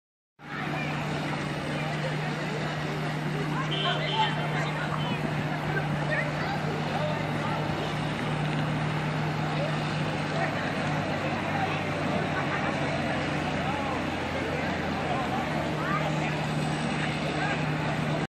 The Merino FFA Color Guard makes its way down the Merino Fall Festival Parade route
By: Callie Jones - Journal-Advocate education reporter
Published on: September 13, 2014